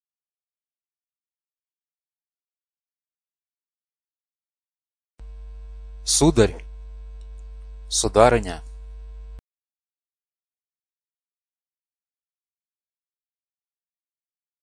How to address correctly in Russian to the unfamiliar woman or man
How to address (refe) correctly in Russian to the other person or persons. Phrases when making a formal introduction. How to introduce myself.